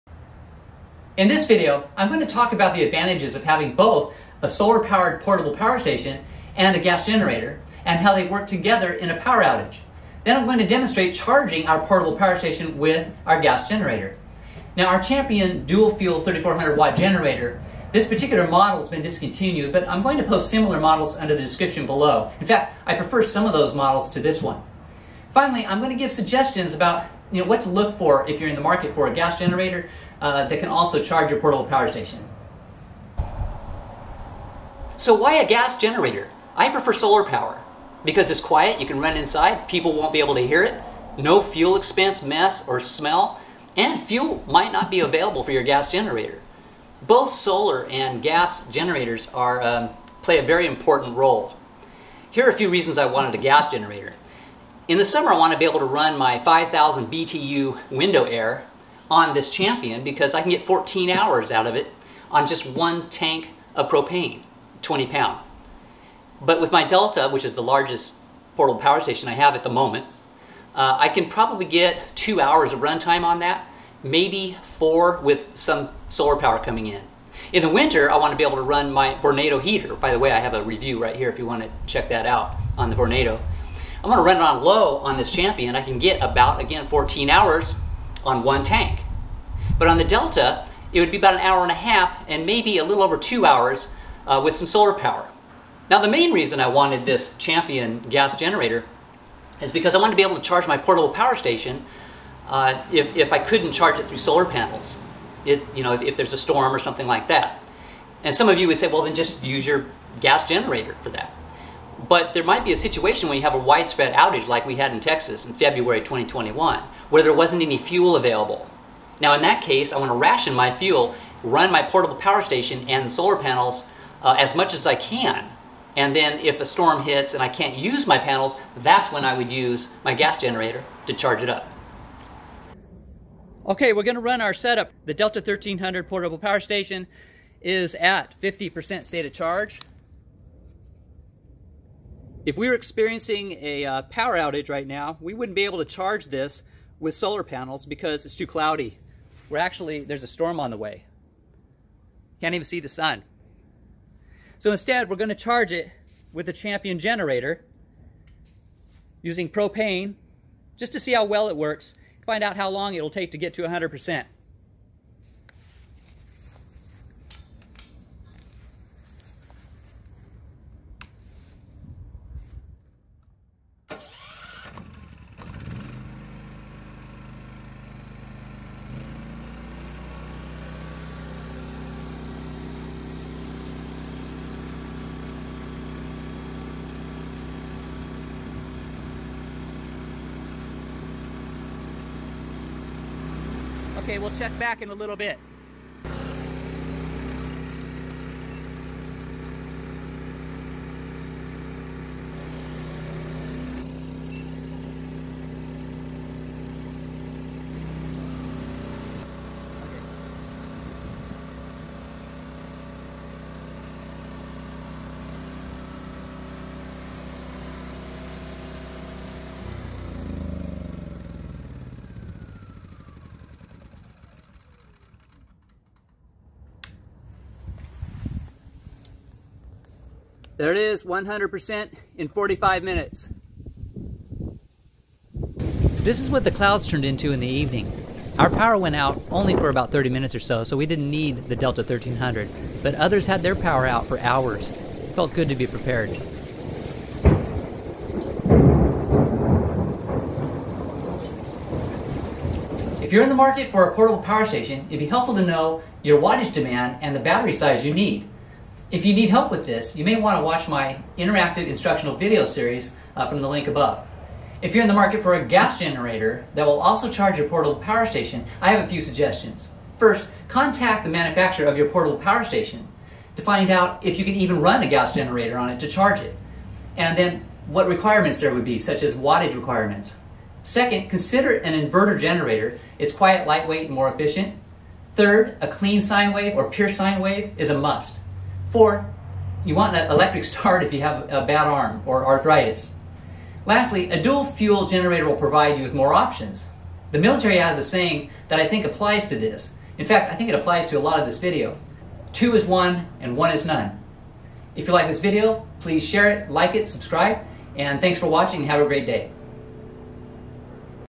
Solar Power Generator and Gas Generator - The Perfect Combination - Charging Solar Gen with Gas Gen
In this video, I explain why it's a good idea to have both a solar generator and gas generator, and I demonstrate charging the solar generator (Delta 1300) with our Champion gas generator.  I also give suggestions on what to look for in a gas generator for this purpose.

Ecoflow Delta Portable Power Station
Ecoflow River Pro Portable Power Station
Ecoflow River Portable Power Station

Champion Power Equipment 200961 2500-Watt Dual Fuel Portable Inverter Generator, Ultralight (This is on Amazon and is good jumping on point to see other Champion Inverter Generators)

I particularly like the 4500-Watt Dual Fuel Inverter 200988 and two of the 2000 Watt Dual Fuel Inverter 100900 generators connected by a parallel kit sold separately.  Be sure to contact your solar generator manufacturer first to be sure it will work okay with you setup.  Also, the 2000 Watt units are not electric start.  Also, Model 100263 and at least another has been discontinued so verify yours is not.  And... Don't forget...  The watt rating you see on the name is the surge watts while using gas!  The surge watts for propane will be less and the actual running watts on both will be less than that.  Look up the specs first!

First of my 3 part series on how to figure out what you can run on your solar generator and for how long and to size it, please see: